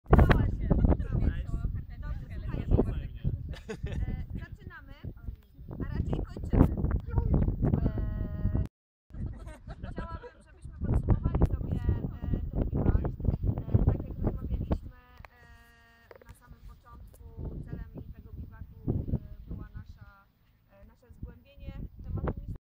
Cogito 2013 - Biwak II, pożegnanie
Wszystko co dobre, szybko się kończy. 3 dni w przepięknym Szczecinku minęły jak oka mgnienie. Więcej o magii Cogito niebawem na